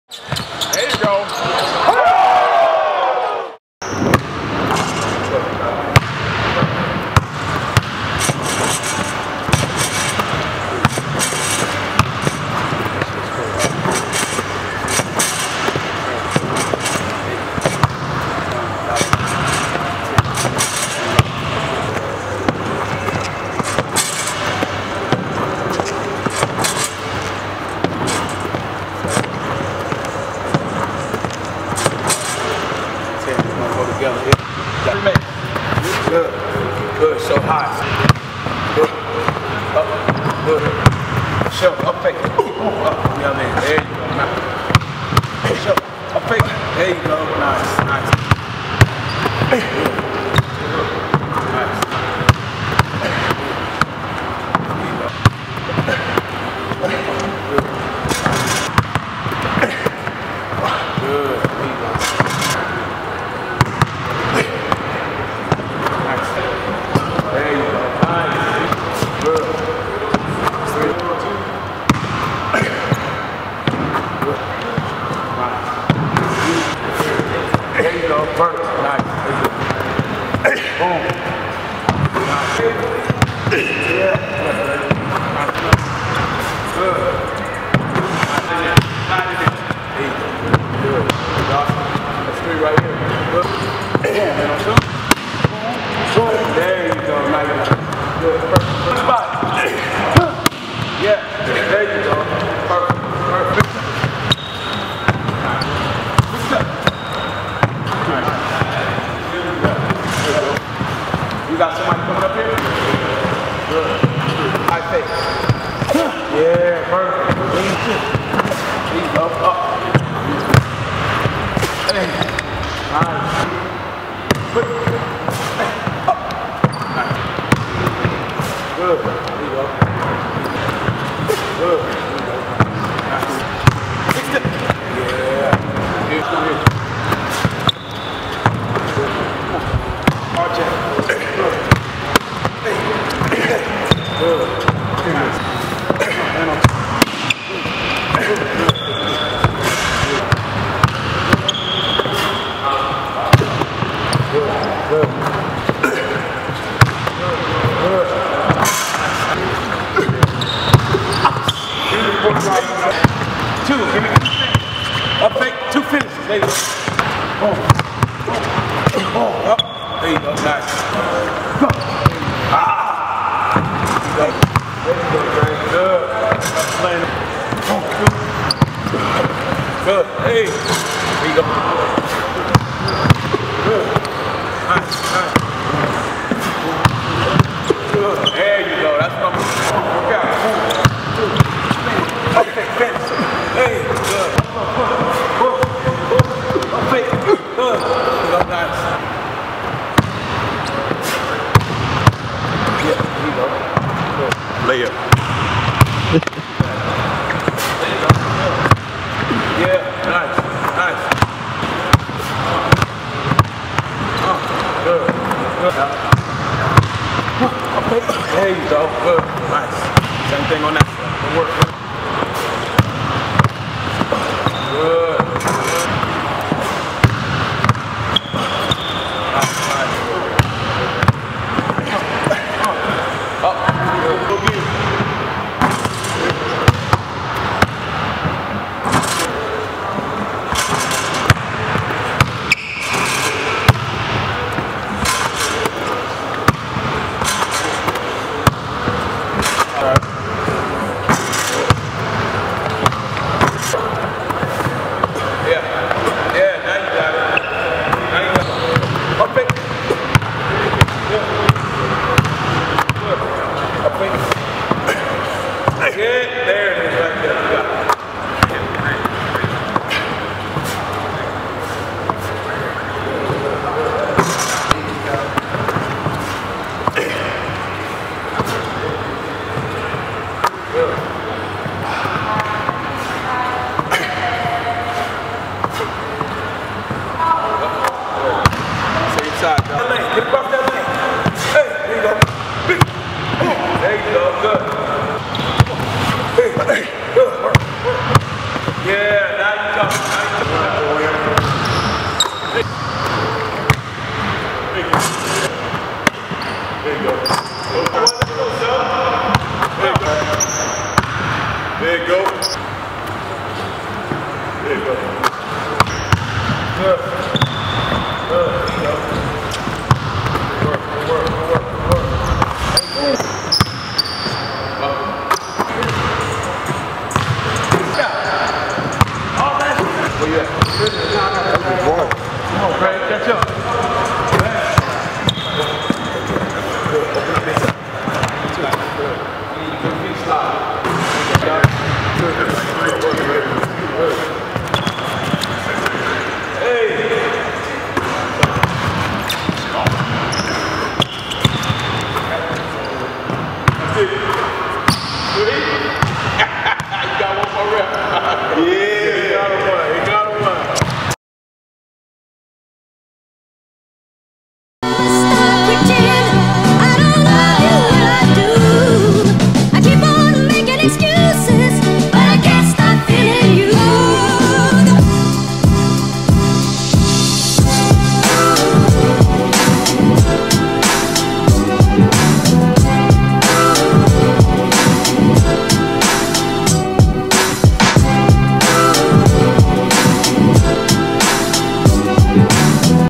Andre Drummond Goes To Work With RemyWorkouts!! | Pistons Center Is A Future NBA All-Star
Andre Drummond got in the gym with one of the top NBA Trainers in South Florida, Stan Remy, while he was in Miami for a few days. The founder of RemyWorkouts put him through a gauntlet of post moves and various other drills as the Team USA member prepares for his fourth season in the League.